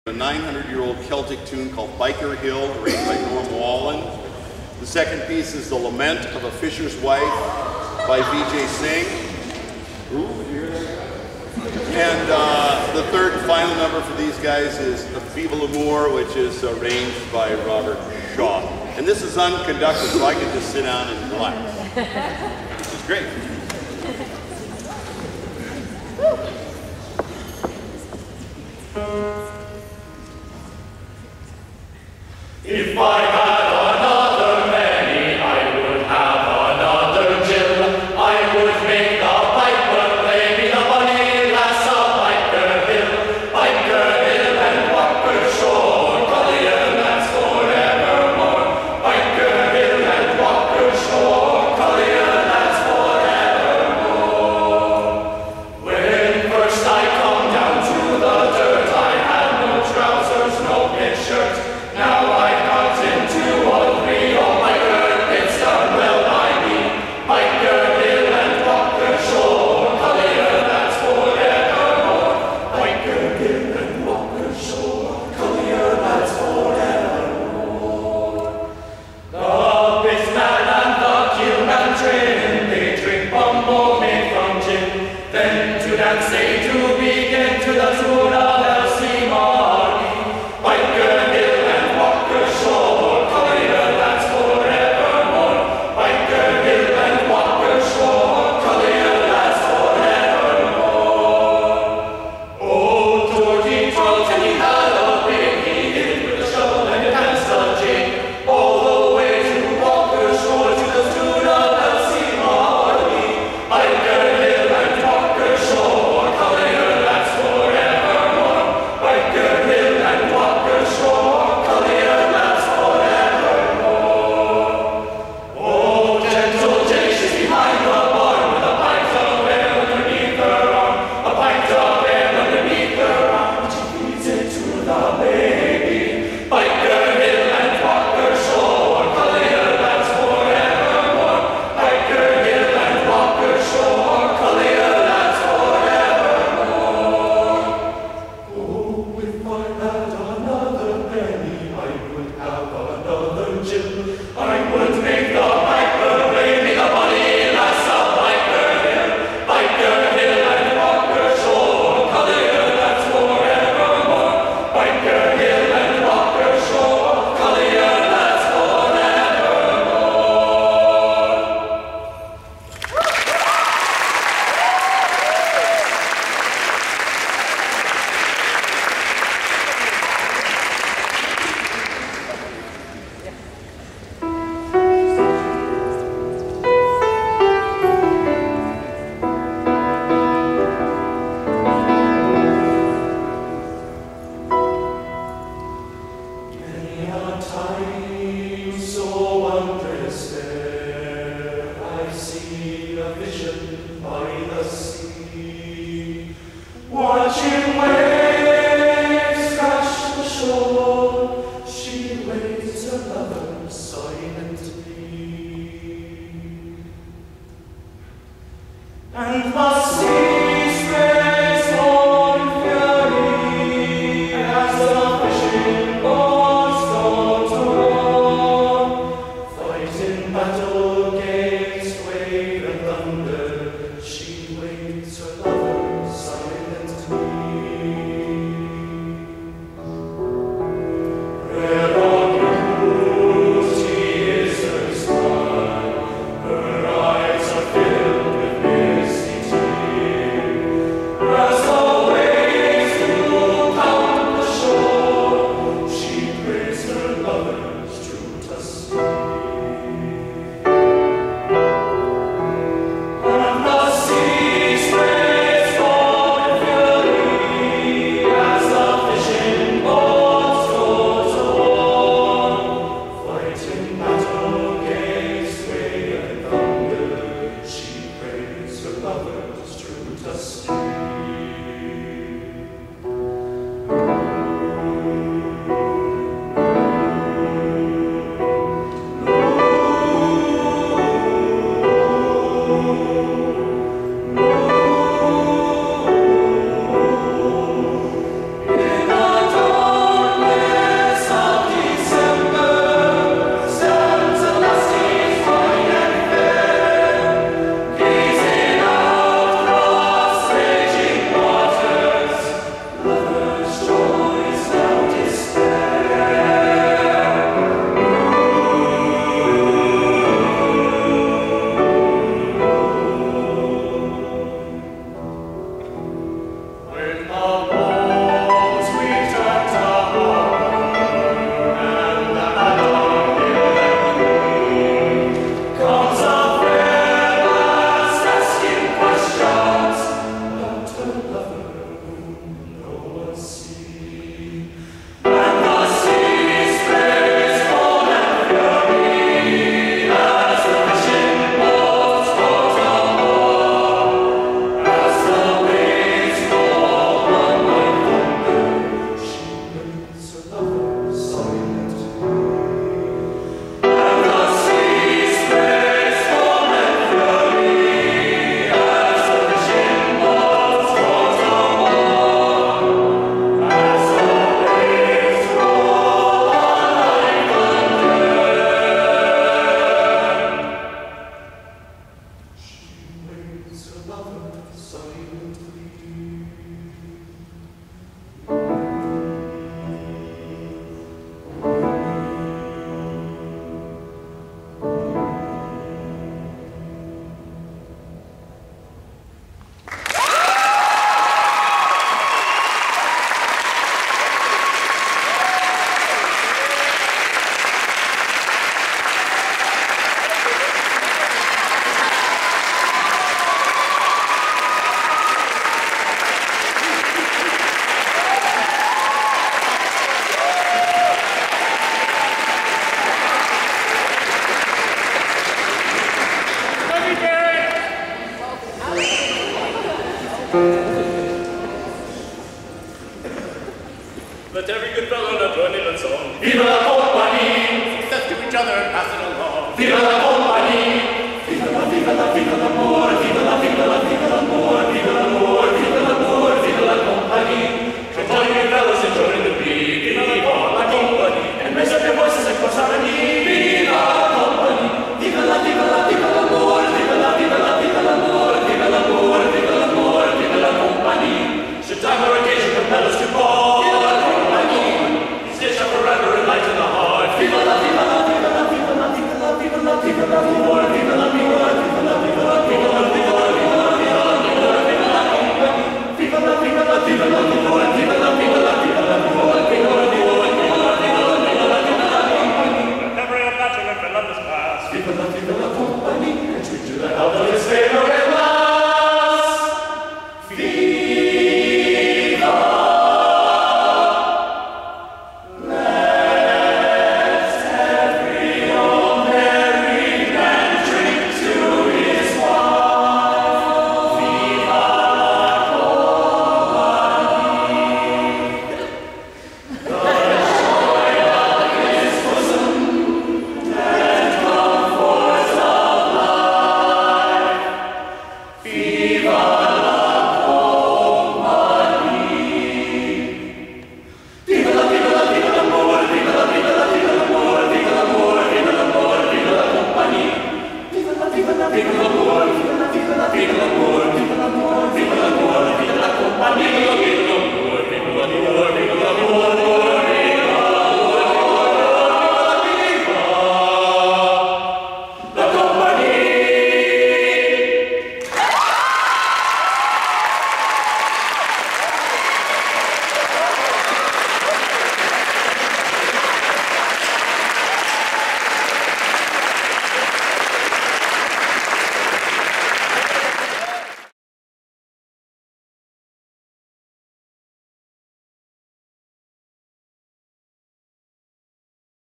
2011_05_26 UHS Select Men's Ensemble - Kamiak HS Exchange Concert
UHS Select Men's Ensemble performance at an exchange concert with Heritage High School and Kamiak High School. The exchange concert was held at Holy Redeemer Catholic Church in Vancouver, WA on May 26th, 2011.